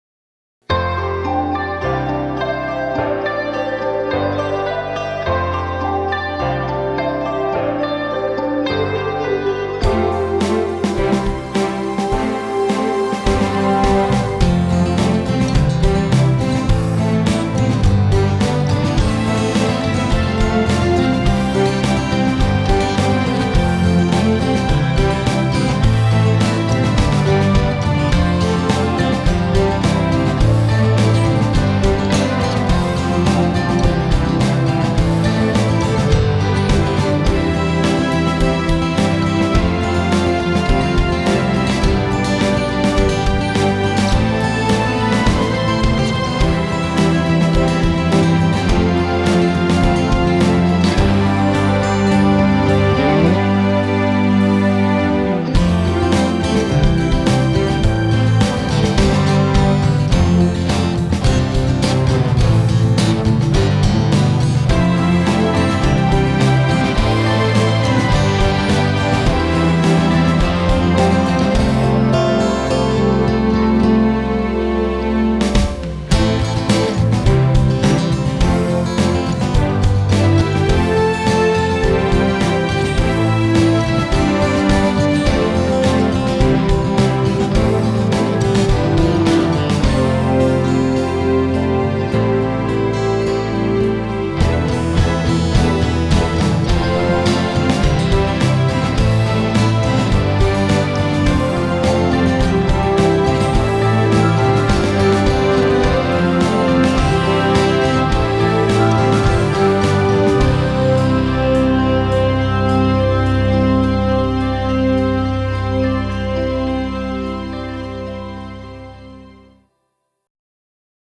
It's Happening Here! Department of Business Newfoundland and Labrador Trade Show Video
Pilot Communications and RMP Productions created several videos to help attract business to Newfoundland and Labrador. This two minute video is intended for trade shows where narration is not required. The original script was illustrated throughout the video to provide context.